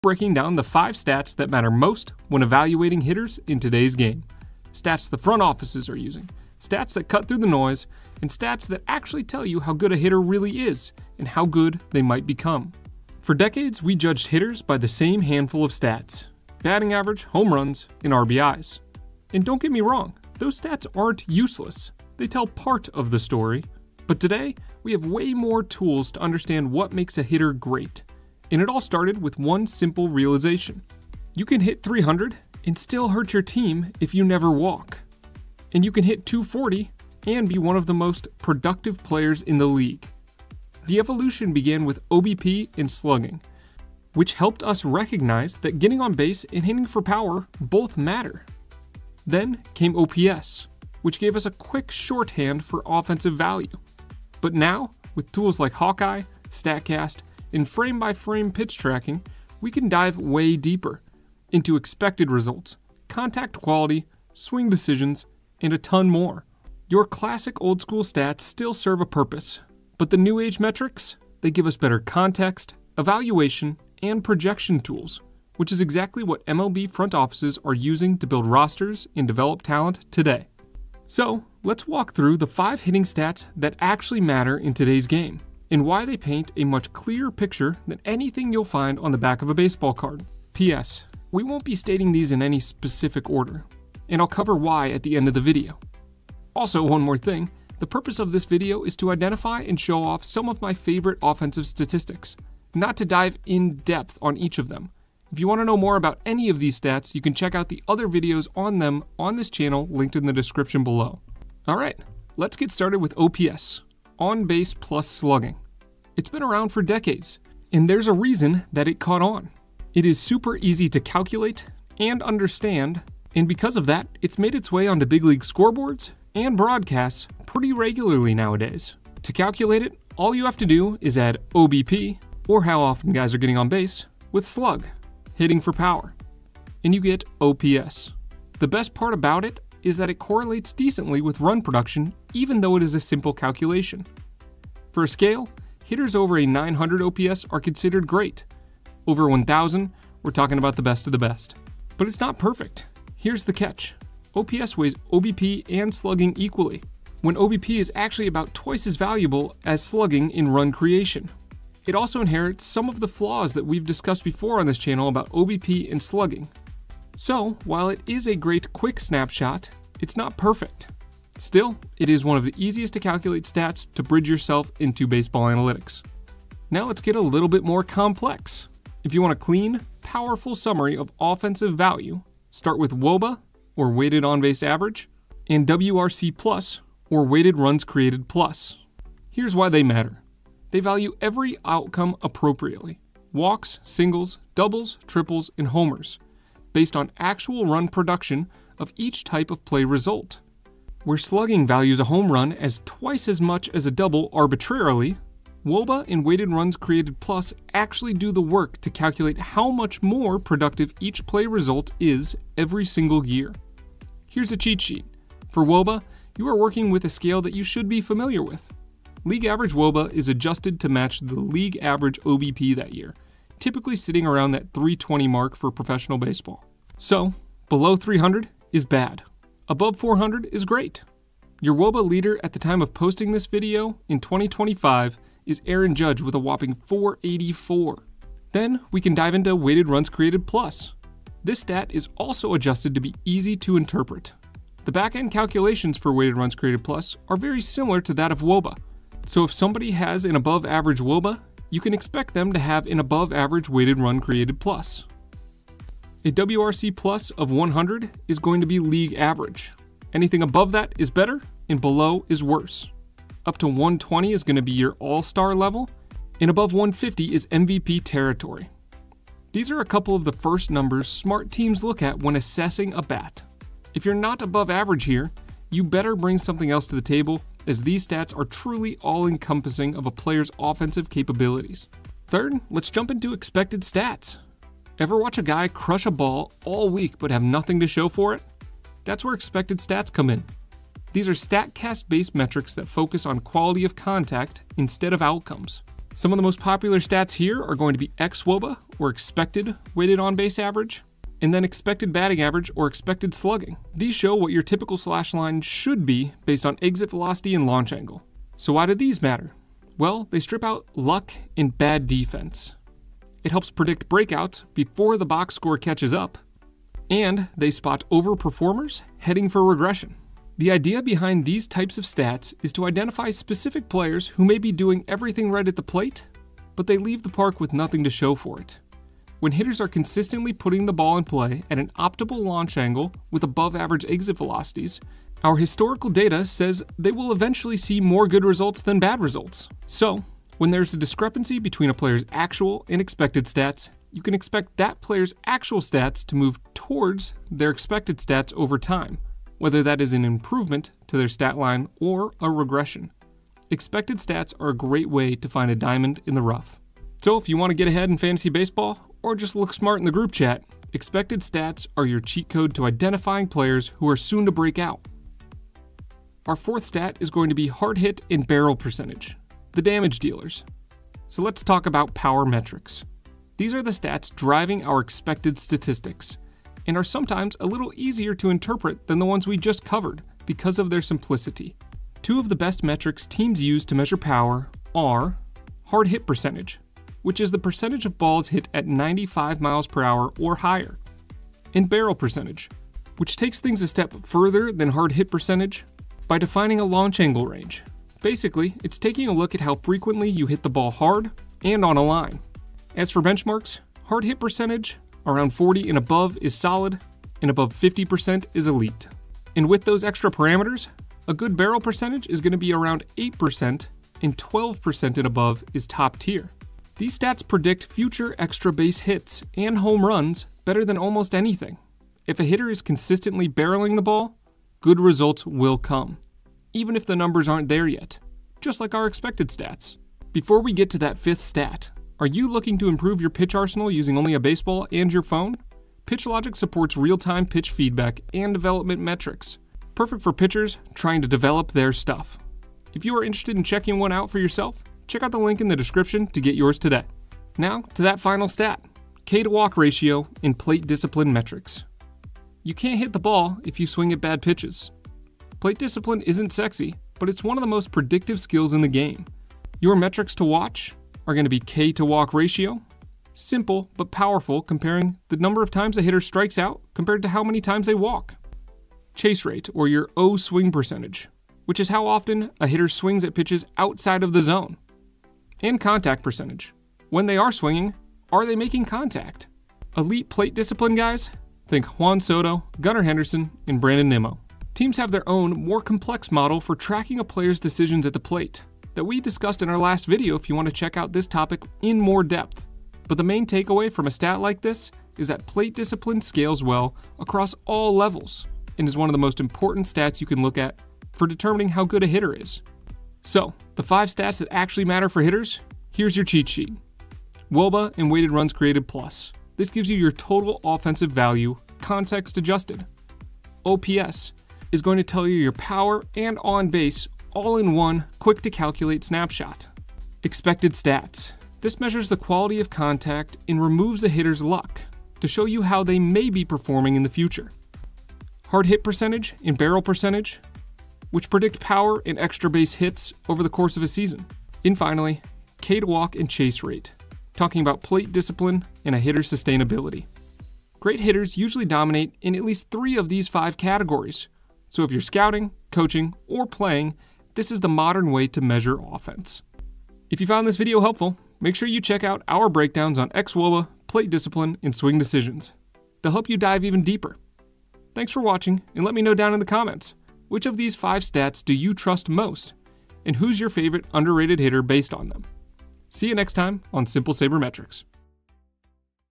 The 5 Hitting Stats MLB Teams Actually Care About
Batting average is out. OPS is just a start.
If you want to understand how MLB teams truly evaluate hitters, you need to know these 5 advanced stats. In this video, we break down the most important hitting metrics used by scouts, analysts, and front offices — including wRC+, xwOBA, Barrel %, and more.

Whether you're a fantasy baseball player, a stat-savvy fan, or just tired of old-school numbers like RBI and AVG, this video will change how you see offensive performance.